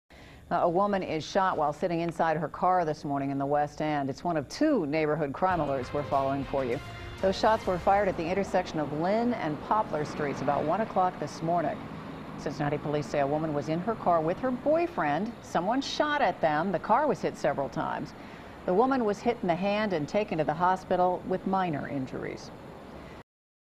Woman Shot Inside Car
CINCINNATI, Ohio (WKRC) -- A woman is shot while sitting inside her car this morning in the West End. The shots were fired at the intersection of Linn and Poplar Streets around one Tuesday morning. Cincinnati Police say a woman was in her car with her boyfriend when someone shot at them.  The car was hit numerous times.  The woman was hit in the hand and taken to the hospital with minor injuries.